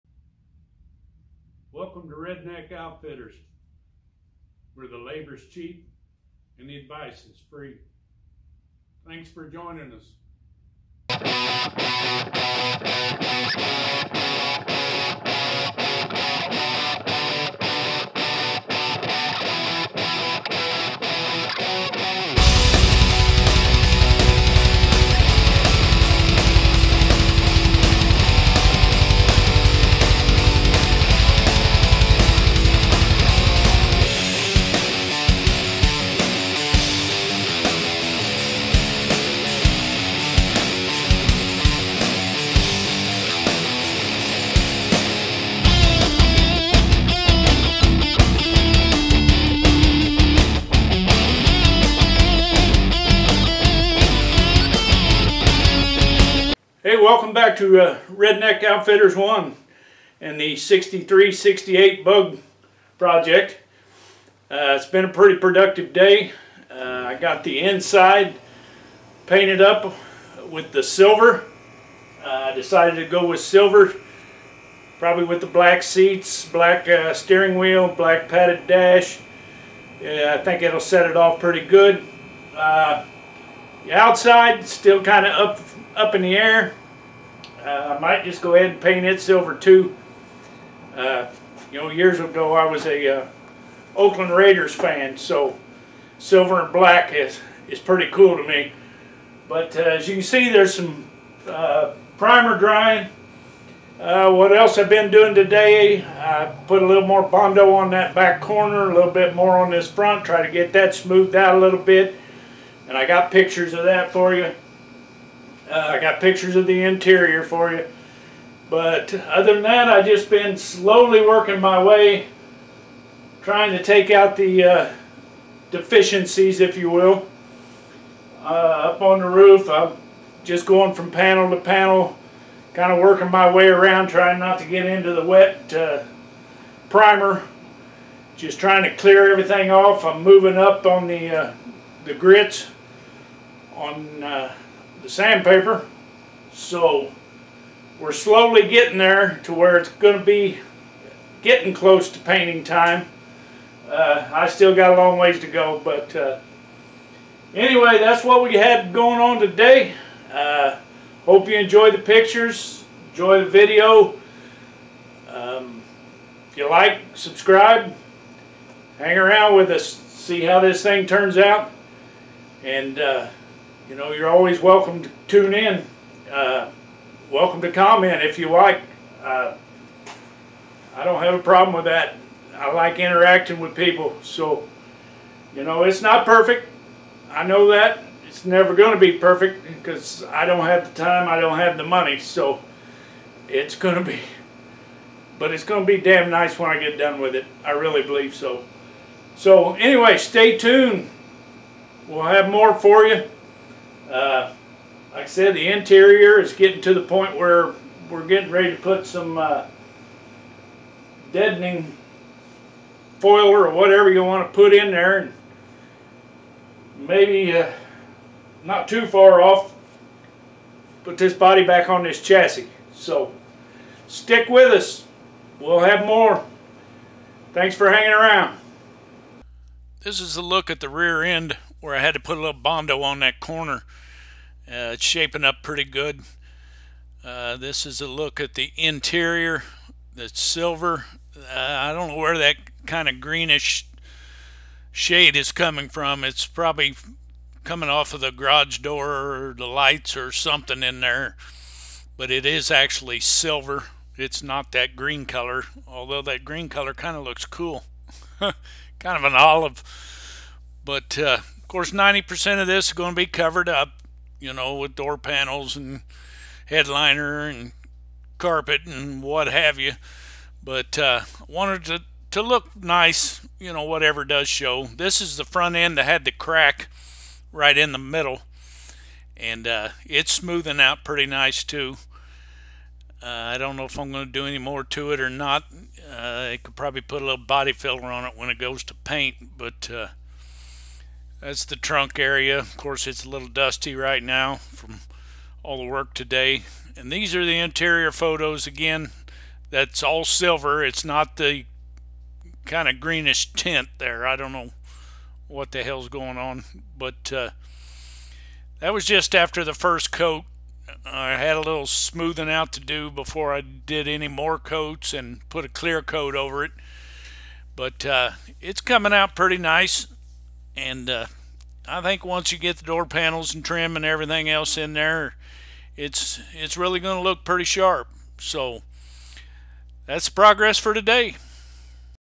63/68 Bug Project Interior paint
Update during the painting of the interior on the63/68 Bug Project